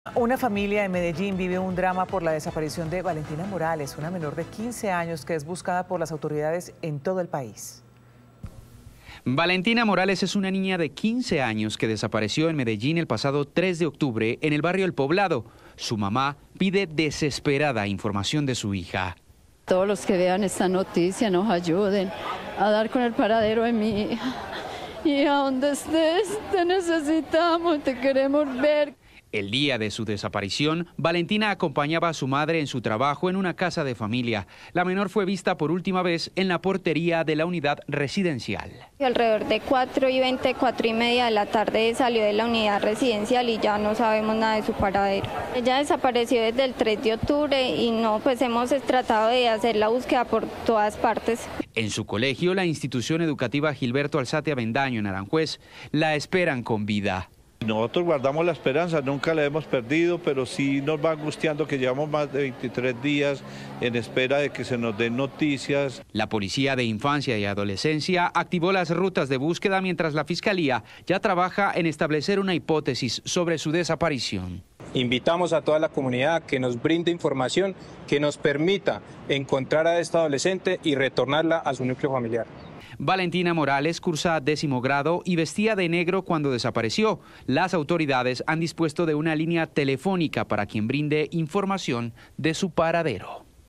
Angustiosa búsqueda de Valentina Morales, adolescente que desapareció en Medellín
Una familia vive un drama por la desaparición de Valentina Morales, una menor de 15 años que es buscada por las autoridades en todo Colombia.

Valentina Morales es una menor de edad que desapareció en Medellín el martes 3 de octubre de 2023 en el barrio El Poblado. En medio de angustia, su mamá clama información que permita encontrarla.

“A todos los que vean esta noticia, por favor que nos ayuden a dar con el paradero de mi hija. Donde estés, te necesitamos, te queremos ver”, indicó Fabiola Morales, madre de la menor desaparecida.